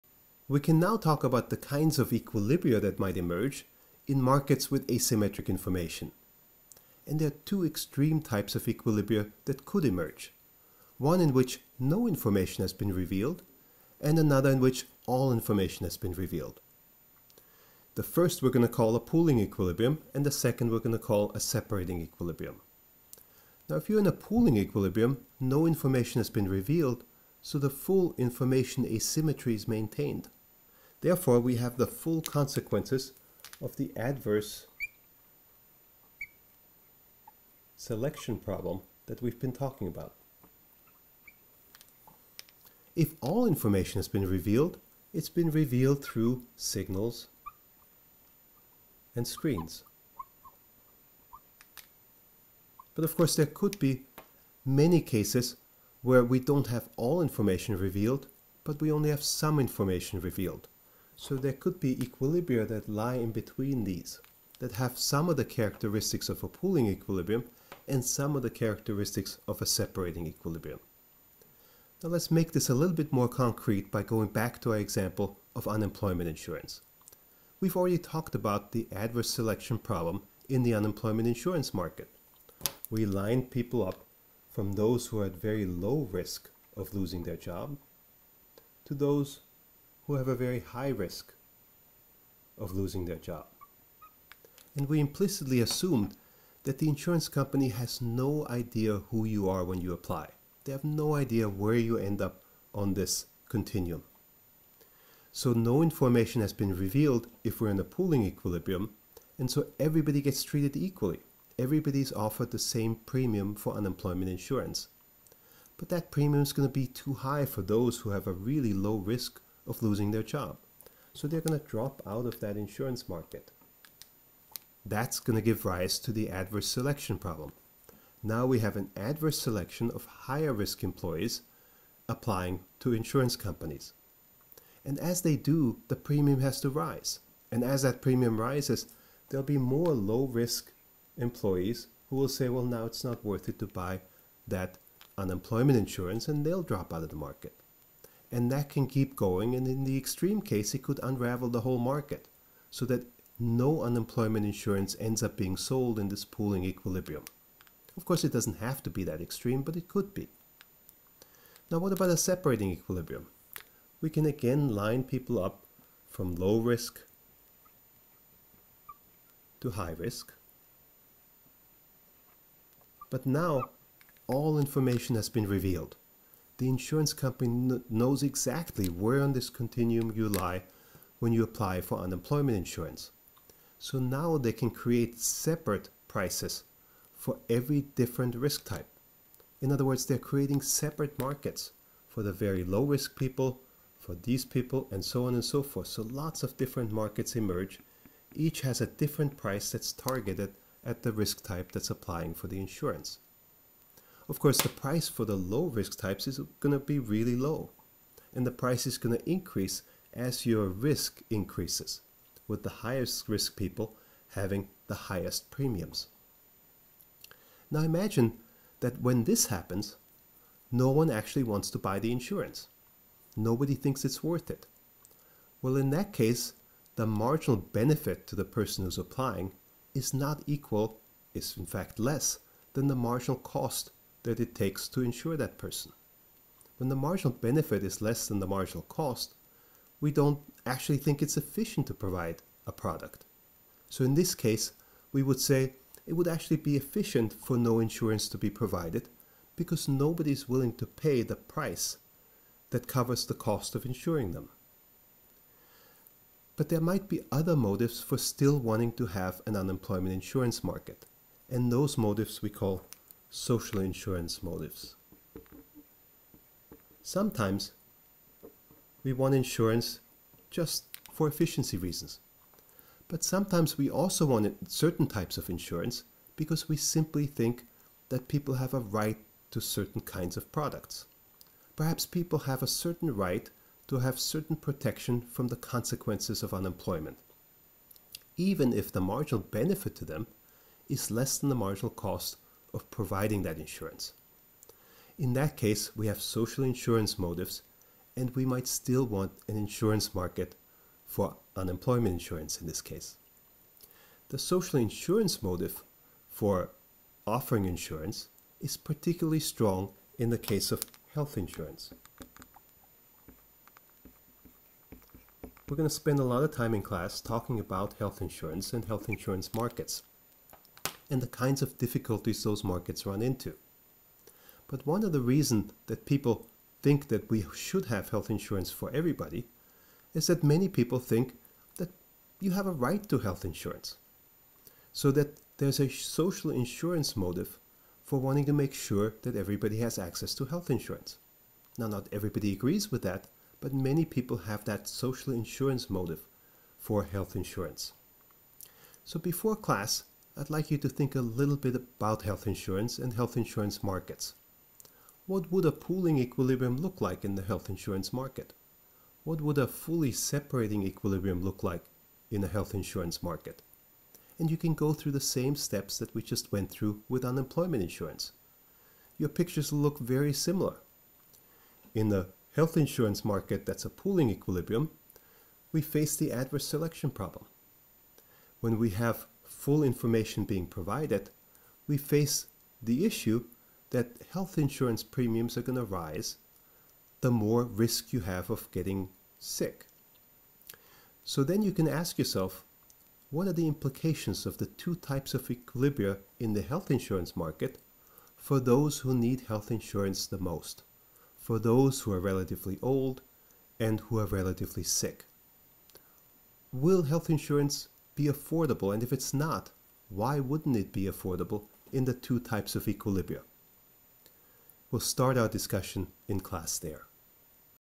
14.3 Equilibria with Asymmetric Information
This video introduces the idea of a pooling equilibrium -- where no information has been revealed and the information asymmetry persists in the market, and the idea of a separating equilibrium -- where information has been fully revealed through signals or screens. It then asks you to think about how these ideas relate to issues that emerge in health insurance markets where social insurance motives cause people to want everyone to have access. We will explore health insurance markets and policies in more depth in class.